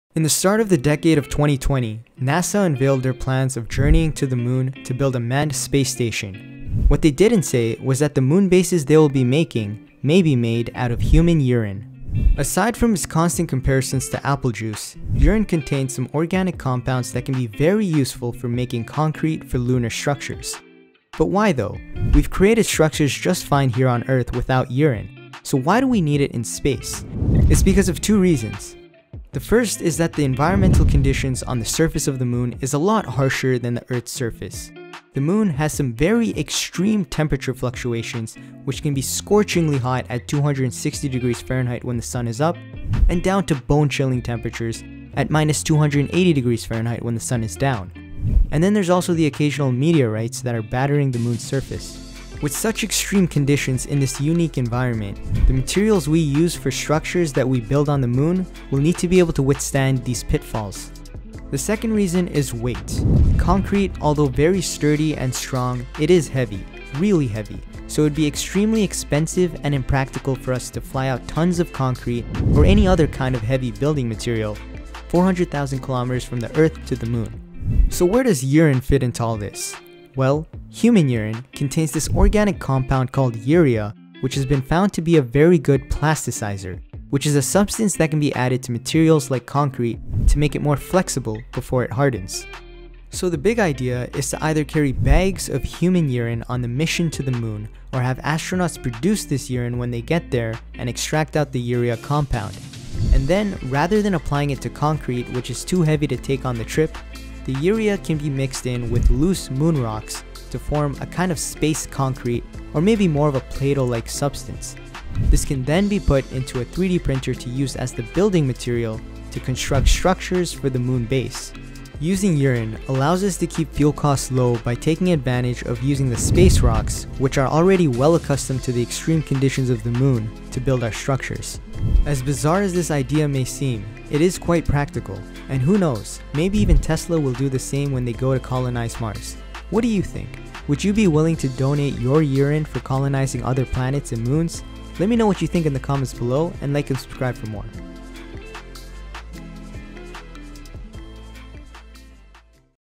Astronauts May Use Human Urine To Build Moon Base
Astronauts plan on building a manned space station on the moon, which they may be building using their own pee!